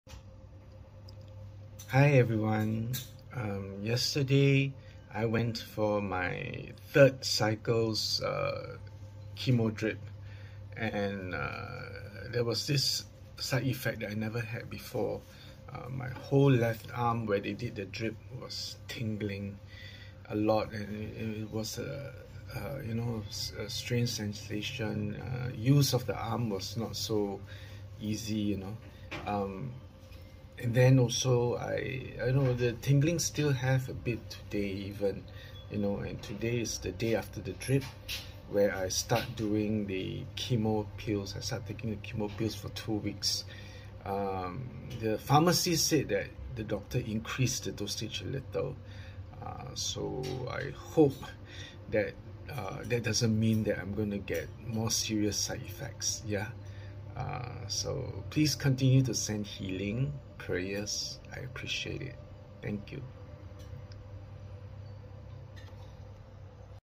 3rd chemo cycle. Side Effect - arm tingling
Hi everyone,
This is my chemotherapy treatment update.
I am on the 3rd cycle now.
Had the chemo drip yesterday.
And experienced a side effect - my left arm, where the drip was, tingled.
It made using the arm a little difficult.
The next day, my arm was still tingling. But less.

If you are interested to earn side income - Join My Private FB Group- Teaches You Skills That Help You Earn Side Income 👇

Please continue to send me prayers, healing and good thoughts.

For now, please give me your kind support.